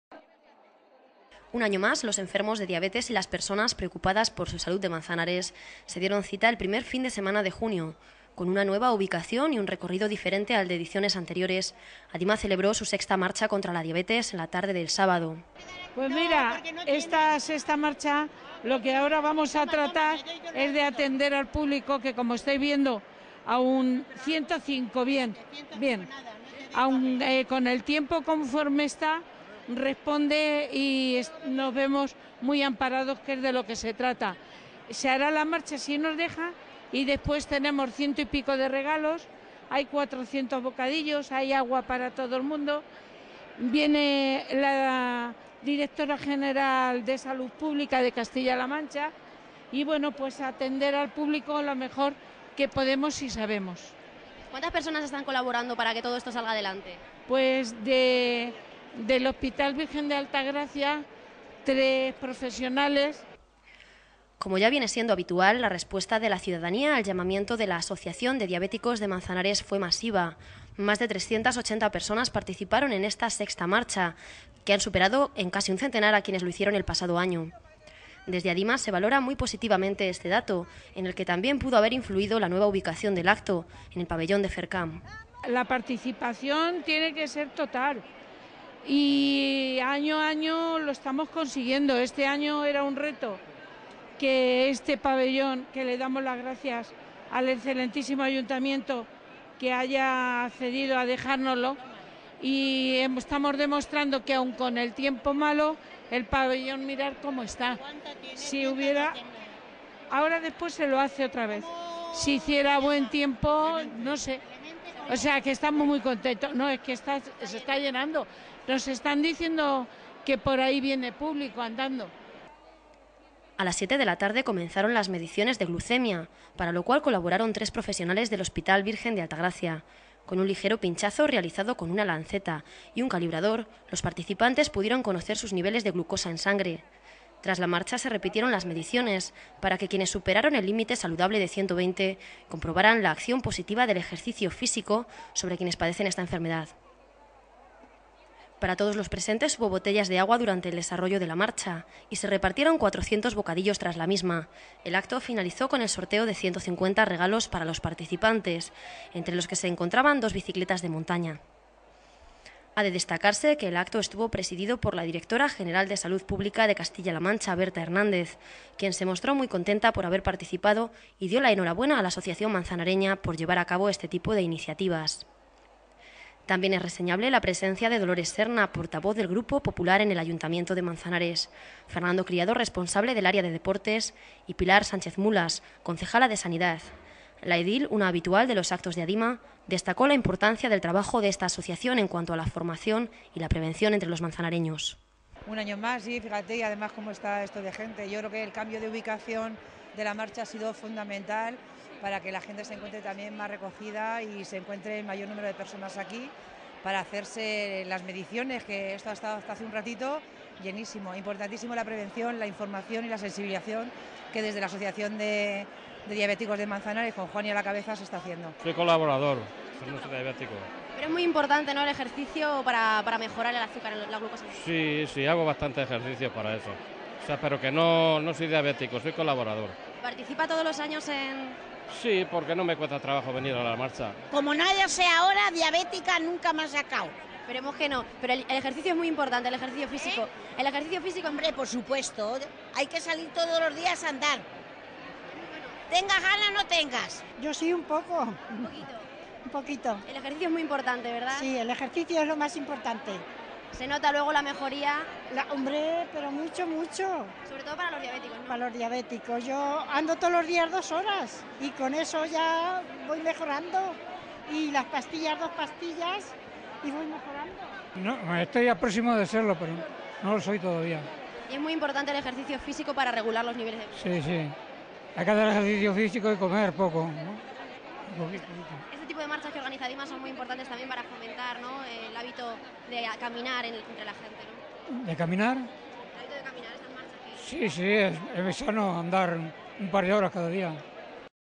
VI Marcha Diabetes
La VI Marcha Contra la Diabetes, organizada por ADIMA, la Asociación de Diabéticos de Manzanares, congregó a más de 380 personas en el Pabellón de FERCAM. Tras las habituales mediciones de los niveles de glucemia a los participantes, comenzó una marcha que rodeó la isla verde y finalizó de nuevo en el lugar de partida. El posterior sorteo de regalos estuvo presidido por Berta Hernández, Directora General de Salud Pública, que resaltó la labor de educación diabetológica de ADIMA.